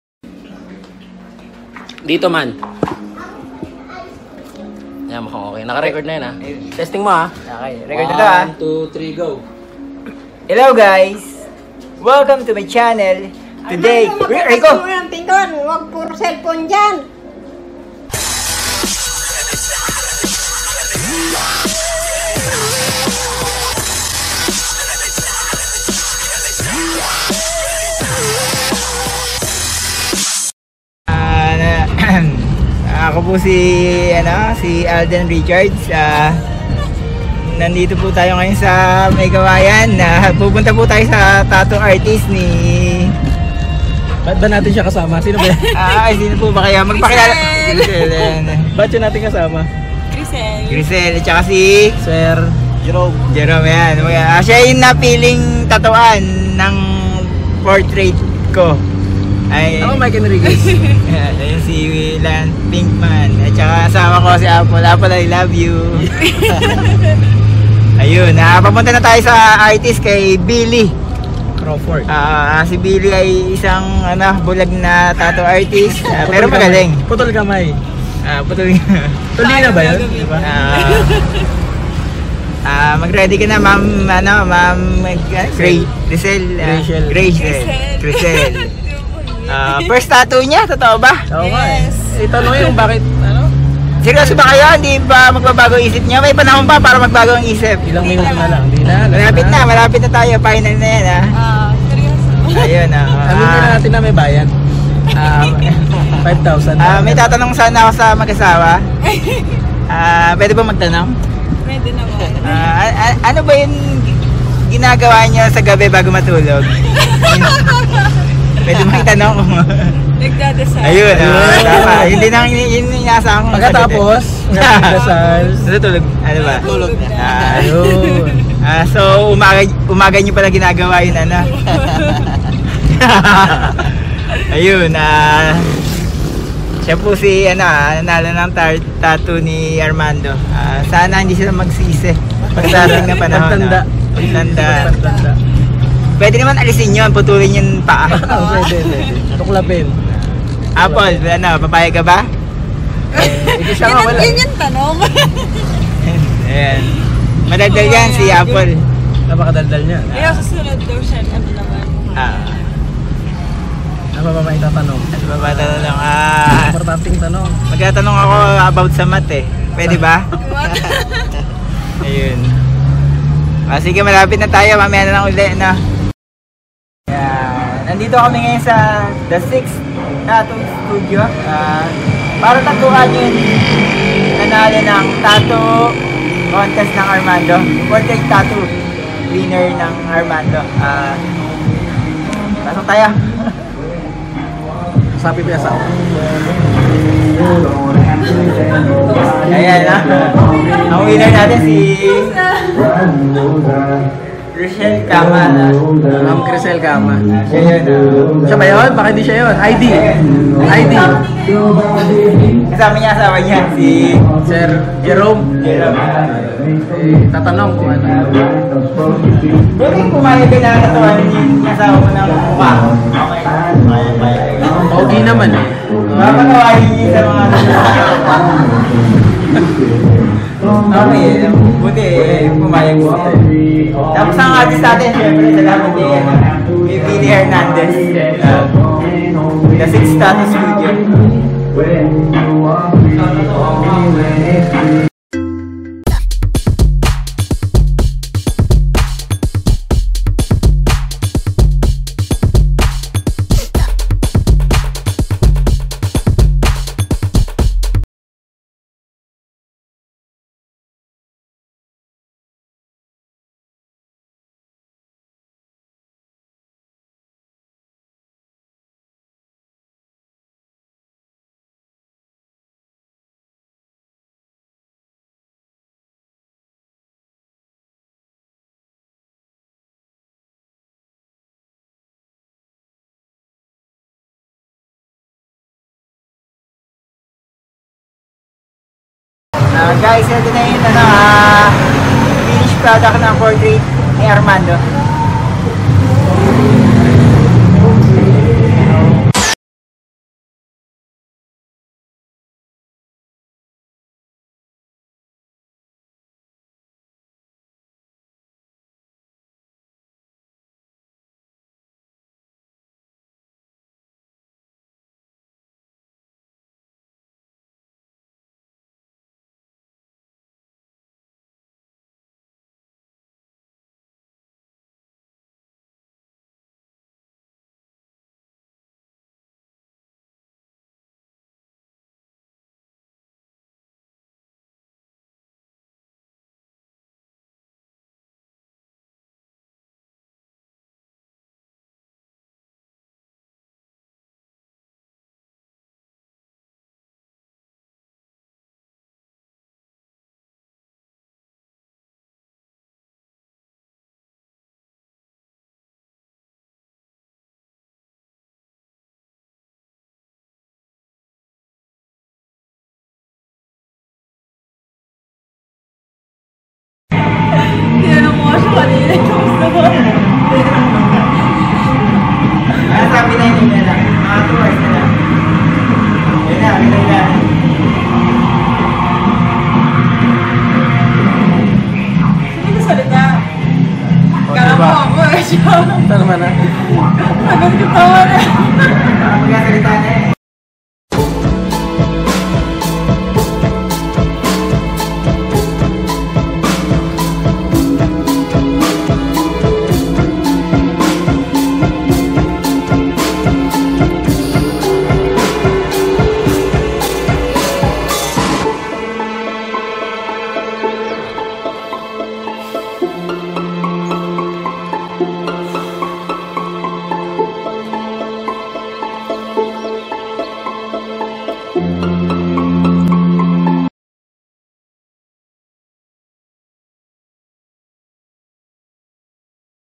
The Winner of Armando Portrait Tattoo!
Nag pa-Premyo po kame ng Free Tattoo sa aming FB page,
kaso ang Tattoo ay Portrait ng muka ko hehe
at heto ang Malas na Nanalo!